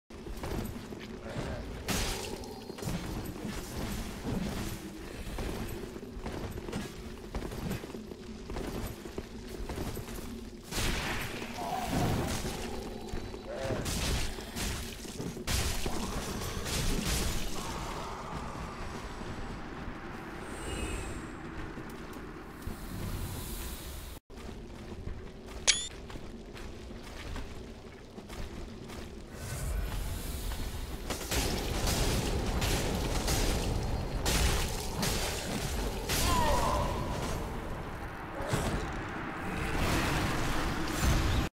turning the tides
After being wailed on for a while I managed to kill the 2 summons and have the host disconnect